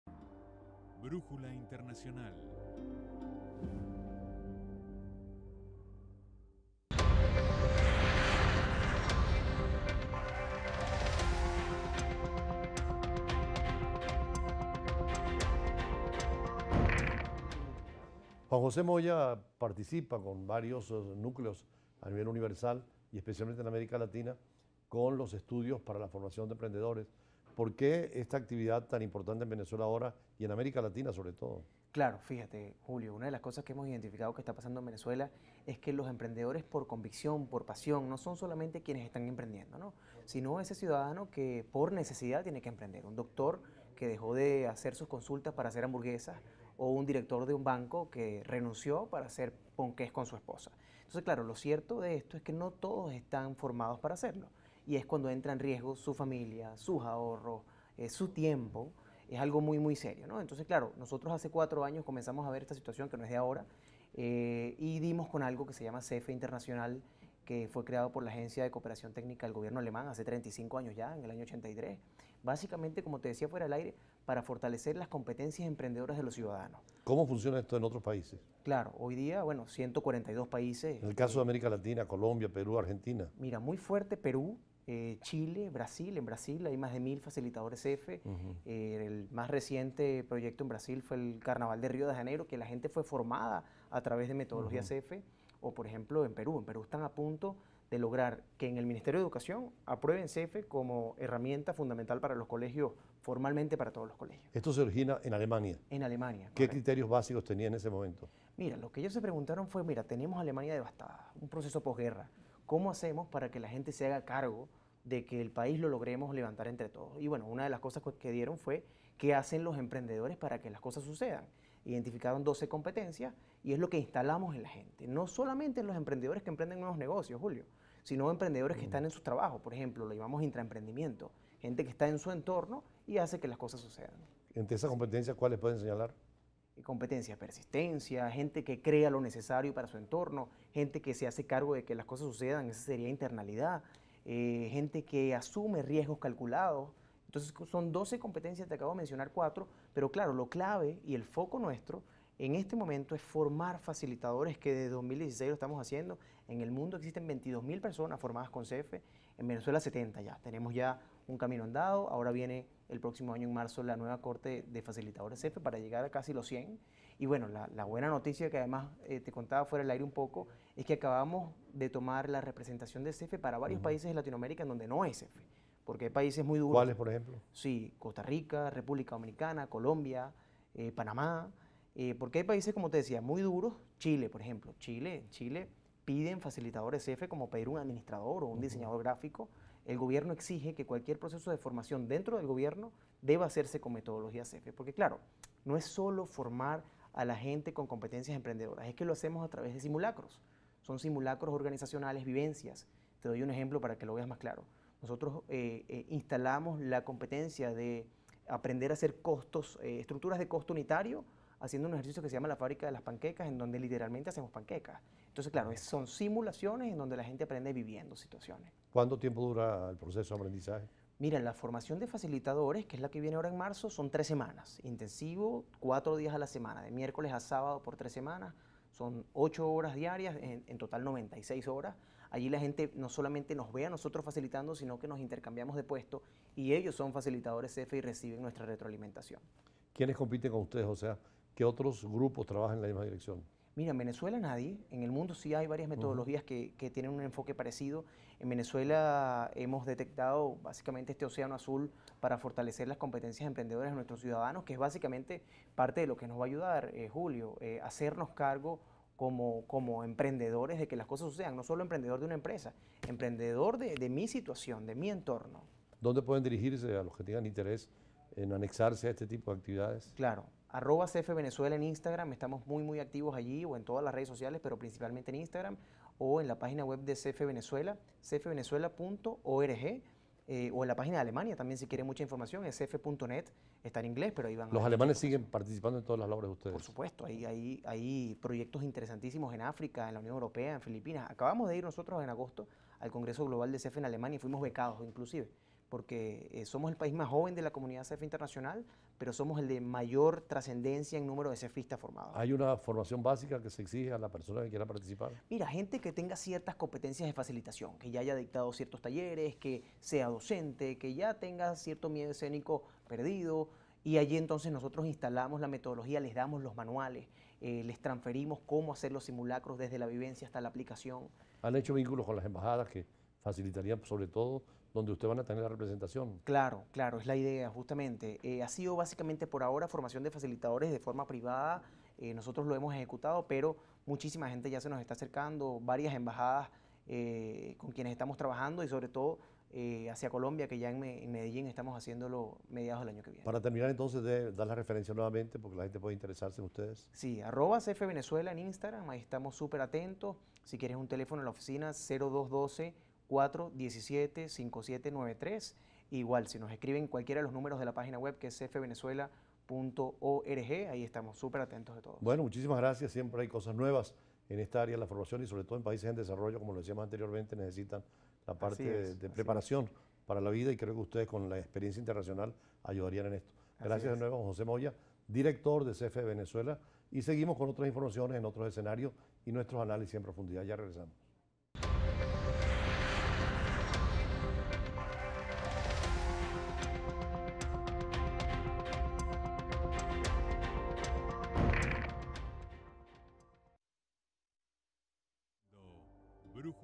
Juan Moya: el CEFE tiene el objetivo de fortalecer las competencias 1-1
El director de Centro de Estudios para la Formación de Emprendedores Venezuela indicó que esta organización nació en Alemania busca brindar herramientas y empoderamiento a quiénes desean emprender.